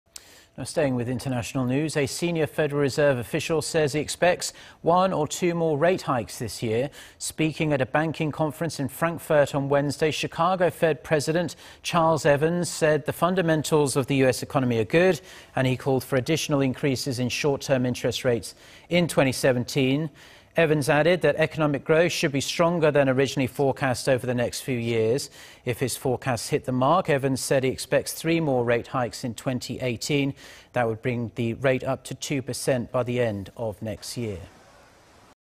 Chicago Fed president supports 'one or two' more rate hikes in 2017
시카고연준총재 "미국 올해 1~2차례, 내년 3차례 금리인상할듯"
A senior Federal Reserve official says he expects one or two more rate hikes this year.
Speaking at a banking conference in Frankfurt on Wednesday,... Chicago Fed President Charles Evans said the fundamentals of the U.S. economy are good... and he called for additional increases in short-term interest rates in 2017.
Evans added that economic growth should be stonger than originally forecast over the next few years.
If his forecasts hit the mark, Evans said he expects three more rate hikes in 2018.
This would bring the federal-funds rate up to two percent by the end of next year.